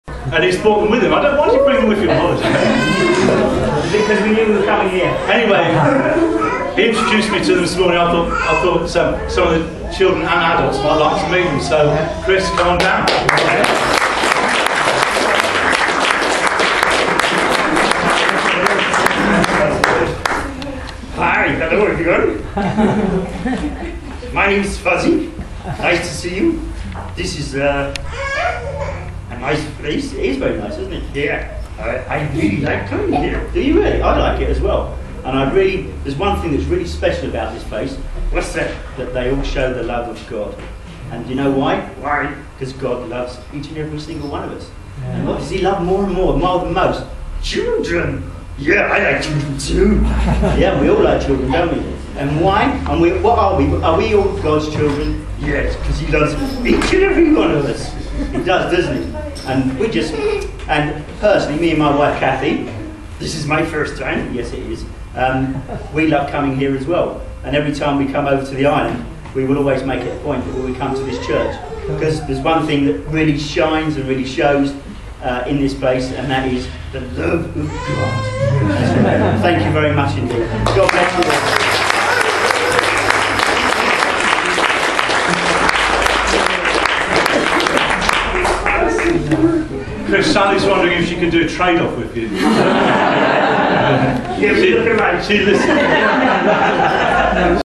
Chris from Essex visits with his puppet friend
Always good to have a puppet visit on a Sunday morning...